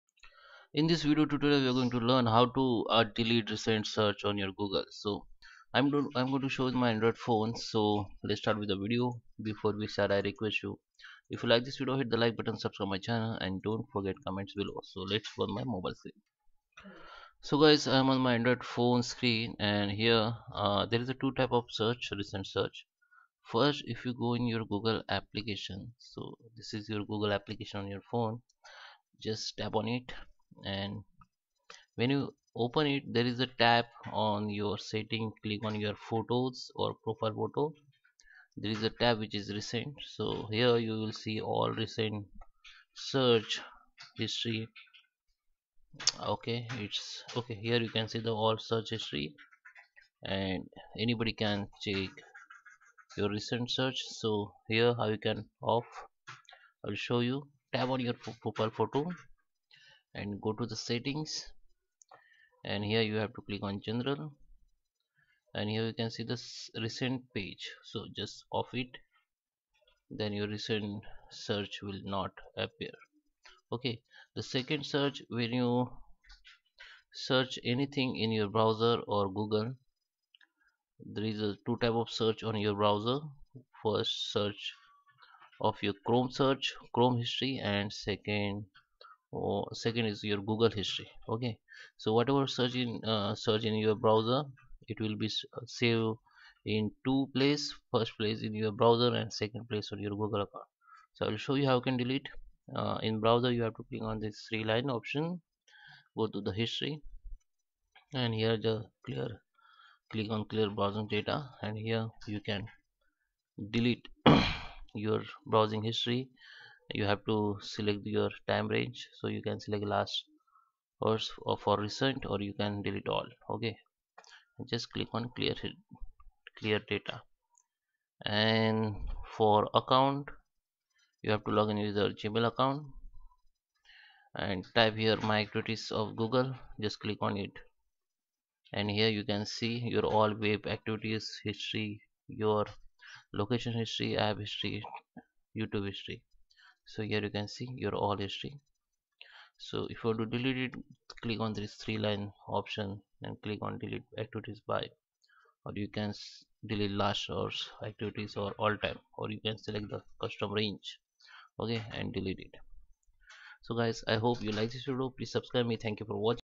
How to Delete Recent Searches On Google
In this video tutorial, we are going to learn, how to delete recent searches on google. follow step by step video to know hot to delete recent google search history.